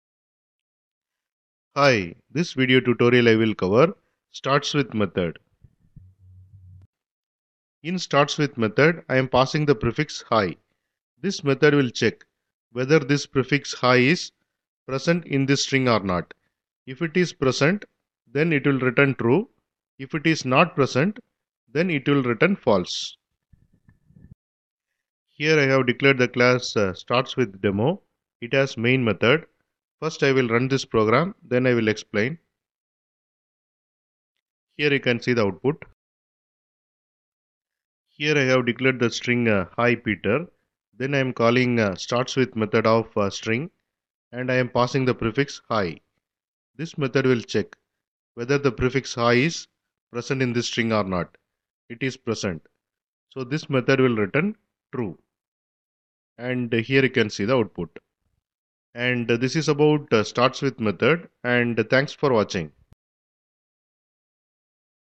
Java String: startsWith(String prefix) Method Explained | Java Tutorial
Welcome to our Java tutorial series! In this tutorial, we'll be exploring the `startsWith(String prefix)` method in Java strings. This method is incredibly useful for checking whether a string starts with a specified prefix. Understanding how to use the `startsWith(String prefix)` method effectively is essential for various string manipulation tasks in Java programming.

In this comprehensive tutorial, we'll cover the following key points:

1. **Understanding the `startsWith(String prefix)` Method:** We'll provide a detailed explanation of what the `startsWith(String prefix)` method does and how it operates within Java strings. You'll learn about its syntax, purpose, and how it enables you to check the beginning of a string.

2. **Practical Examples and Demonstrations:** We'll delve into practical demonstrations of using the `startsWith(String prefix)` method in Java. Through clear examples, you'll see how to effectively call the method on string objects and check whether they start with a specified prefix.

3. **Handling String Beginnings:** We'll discuss various scenarios where the `startsWith(String prefix)` method can be applied, such as checking for specific prefixes, handling file names, or validating input data. You'll learn how to leverage this method to validate and process string beginnings in real-world programming scenarios.

4. **Optimizing String Handling:** We'll explore ways to optimize string handling and improve performance by leveraging the capabilities of the `startsWith(String prefix)` method.

Whether you're new to Java programming or looking to enhance your skills, this tutorial is designed to provide you with a solid understanding of the `startsWith(String prefix)` method and its practical applications.

Java String [startsWith(String prefix) method] | Java Tutorial